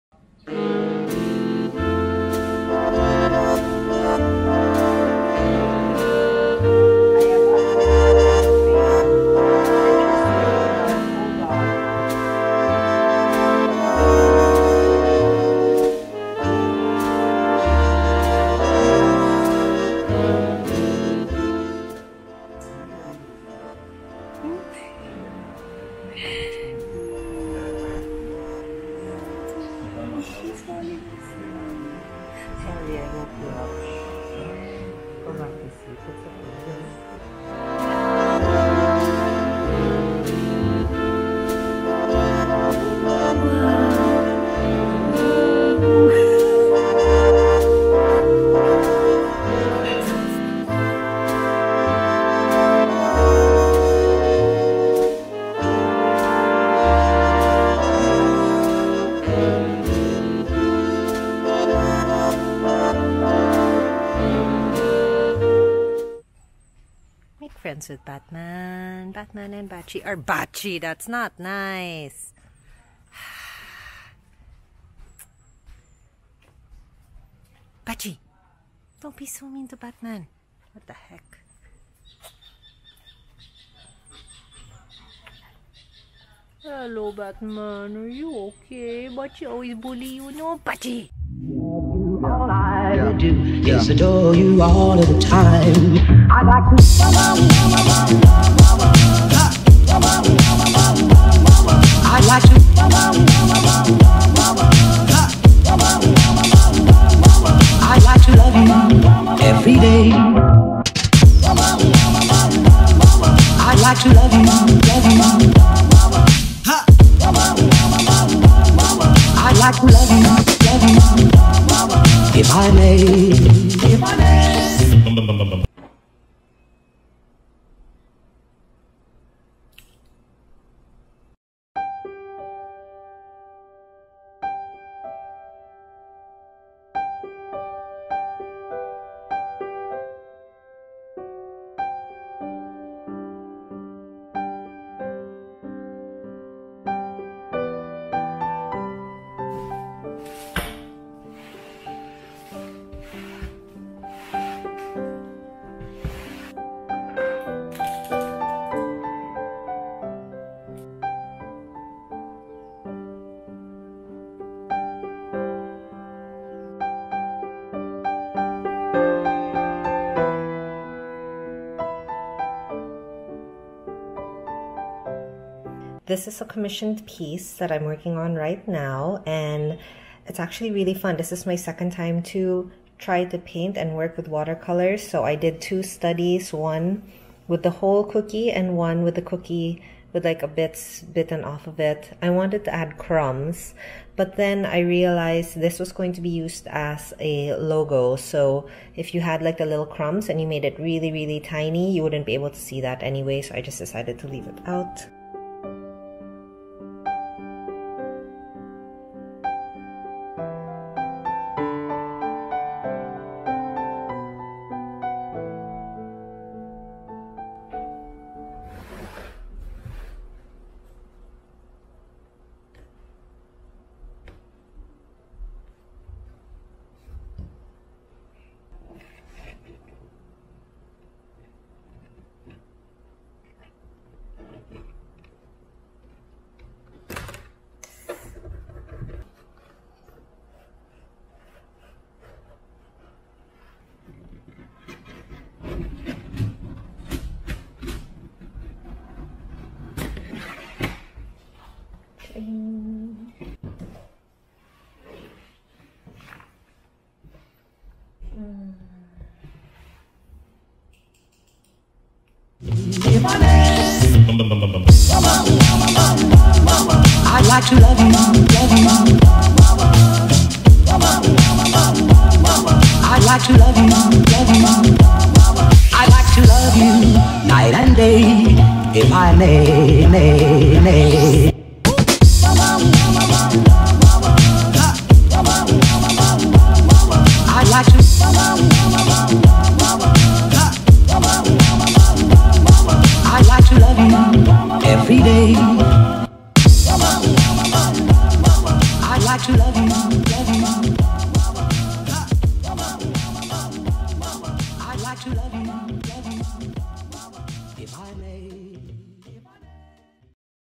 11 Studio Vlog | Catching Up with Friends and Bachi’s Birthday
It’s always good to take time to recharge, to slow down an say thank you for all the things that make life worth living. This week was that for me. I filled up my tank with of love and gratitude. ❤️